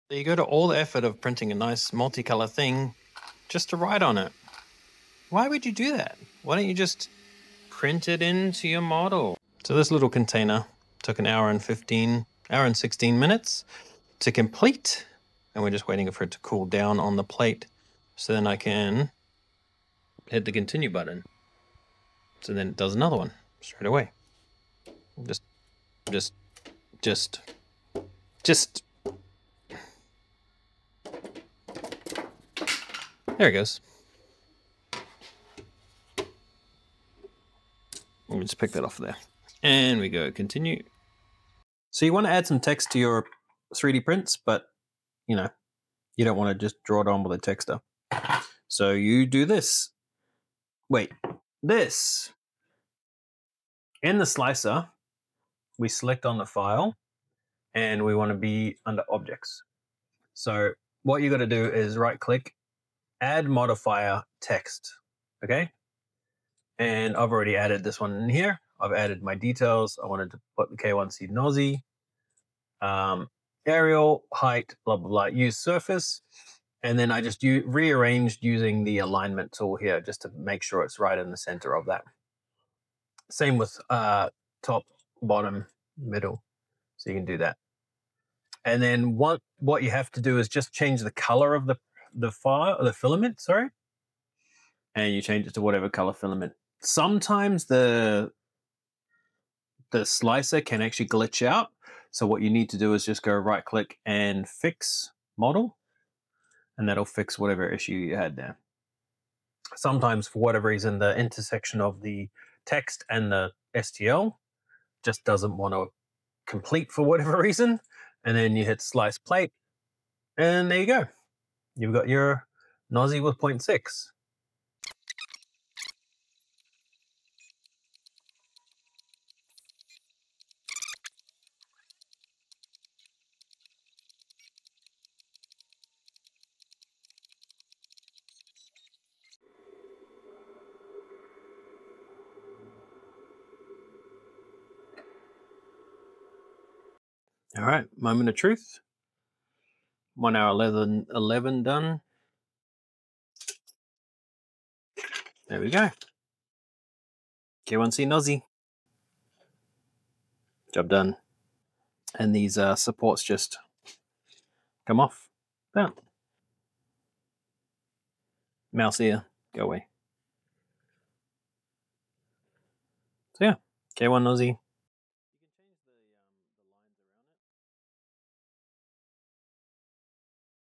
Custom Text in 3D Prints | Flashforge AD5X Multi-Color Tips
Ever gone through the effort of a multi-color 3D print... only to write on it with a pen? Stop that.
In this video, we show how to embed text directly into your prints — no extra tools, no stickers, no shame.

Watch as we prep, print, and finish a text-embedded container using the Flashforge AD5X and OrcaSlicer.

Whether you're labeling parts, customizing gifts, or just flexing your print game — this one's for you.

🔧 Printer: Flashforge AD5X
🧠 Slicer: OrcaSlicer
🎨 Technique: Multi-color with no pauses